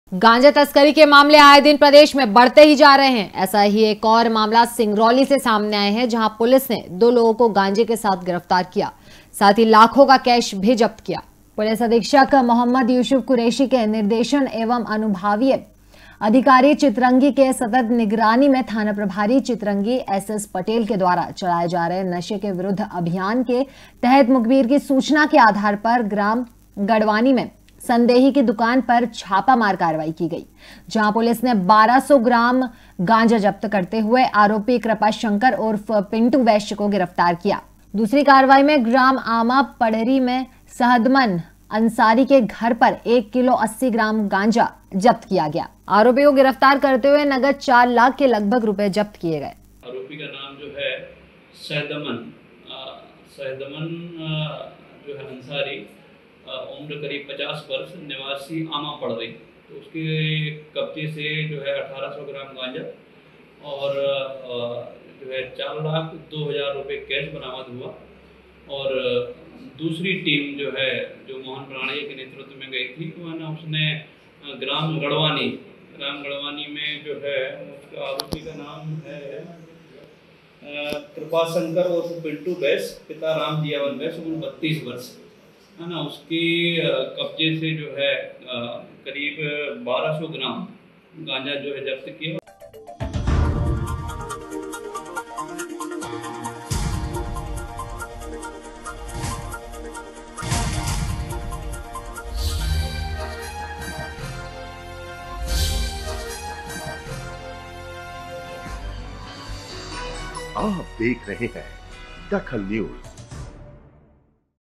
DAKHAL NEWS दो लोगो को गांजे के साथ किया गया गिरफ्तार
दो लोगो को गांजे के साथ किया गया गिरफ्तार
साथ ही लाखो का केश भी जब्त किया गया